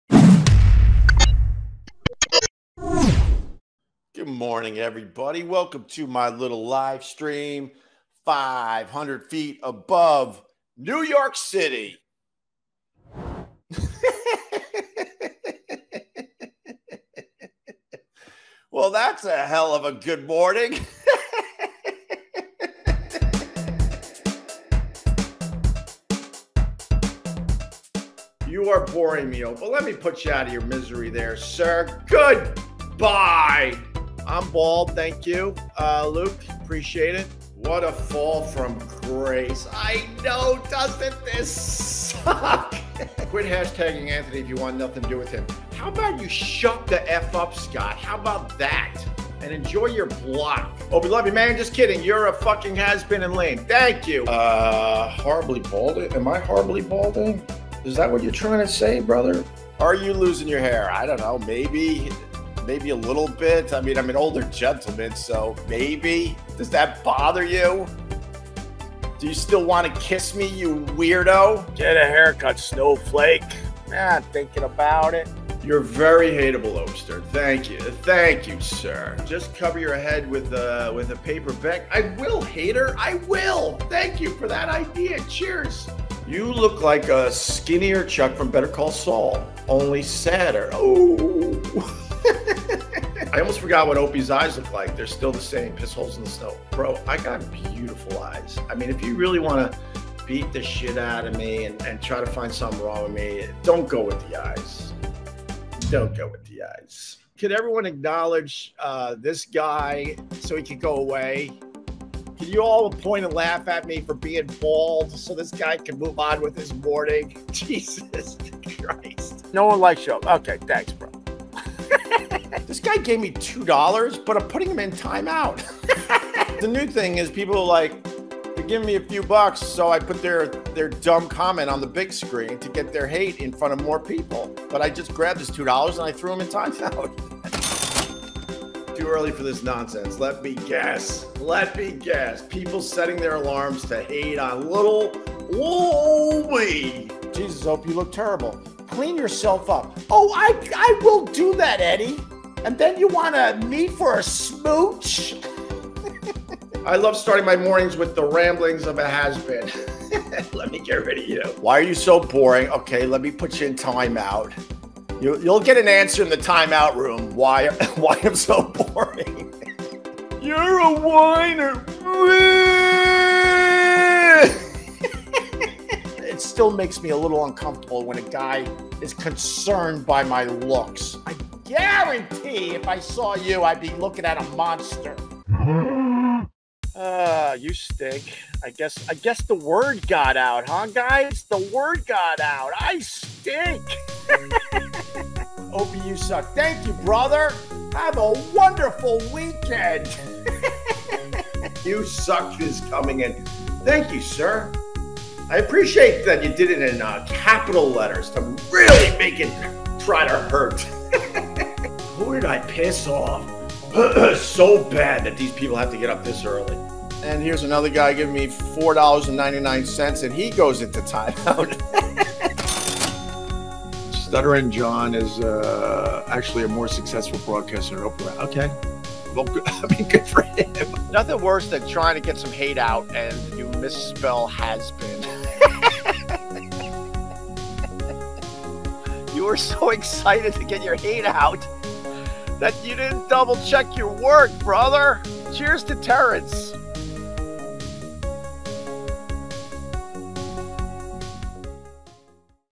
Opie has fun with the haters
During Opie's daily livestream Opie gets the occasional hater that tries to troll him.  Instead of getting mad and blocking them, Opie has fun with the whole thing.  No sign of Gregg Shells here.

Instagram and Tik Tok - OpieRadio